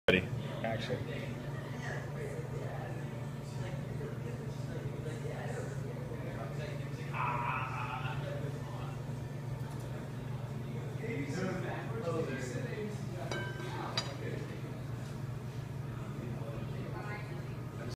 Smoked Sturgeon Headcheese
The reveal of the apple wood smoked sturgeon headcheese at Spoon in Dallas from chef John Tesar.